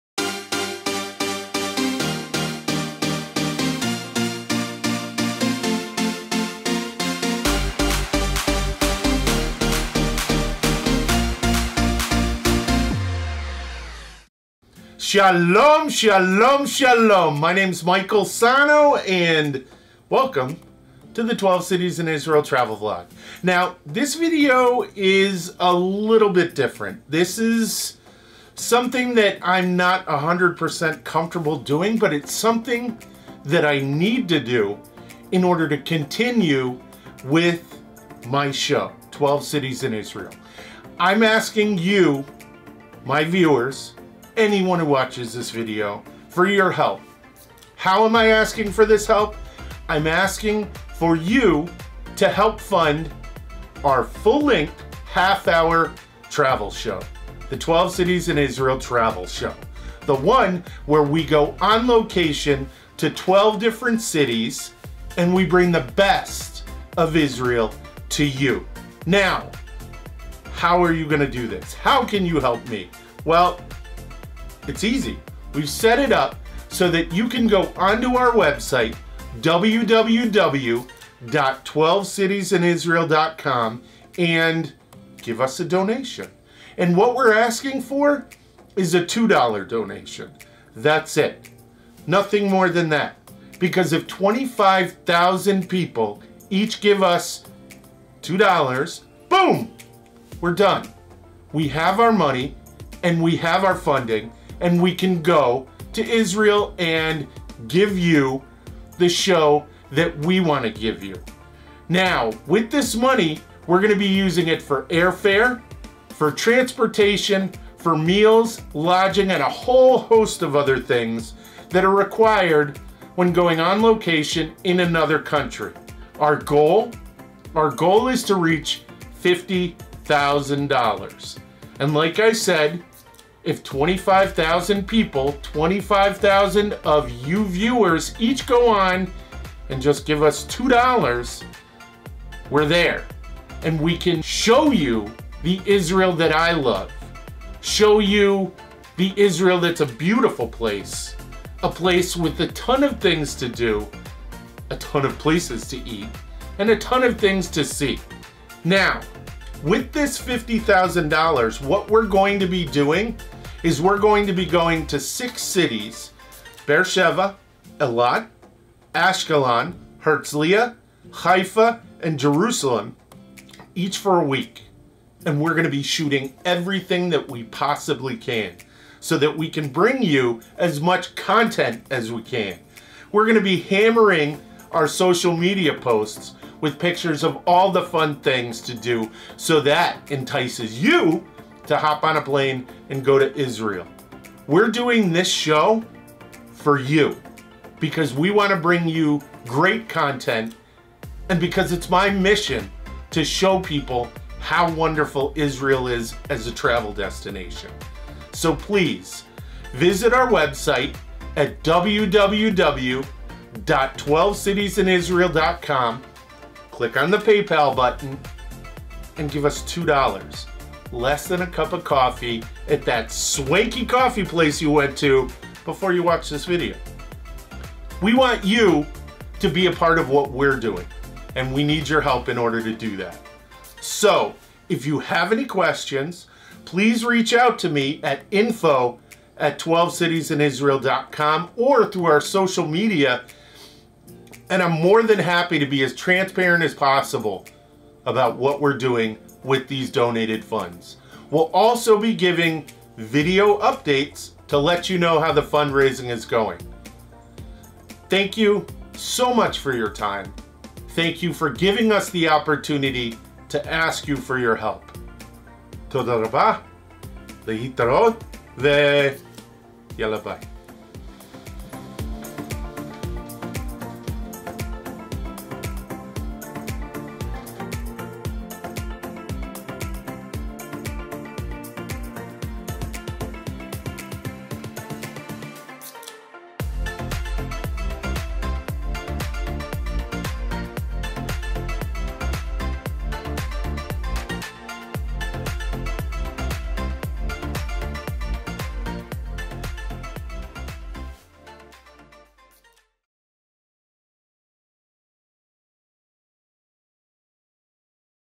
Israel Travel - "12 Cities in Israel" Fundraising Video
Hello Fans, my name is Michael Sahno, the host of "12 Cities in Israel"! We are asking for your help! Please watch this short video to see how you can help us to make our full-length (half-hour) episodes in Israel! to make a small $2 donation toward our filming budget. Help us bring this show to life, so that we can bring Israel to you! And for all who have helped, we thank you from the bottom of our hearts! תודה רבה!!!

Travel to Israel and see the food and the fun with 12 Cities in Israel.

Host:
Michael Sahno

Produced by:
Michael Sahno & Rachel Villegas

Cinematographer:
Rachel Villegas

Associate Produce & Social Media Director:
Miriam Strauss

Video Info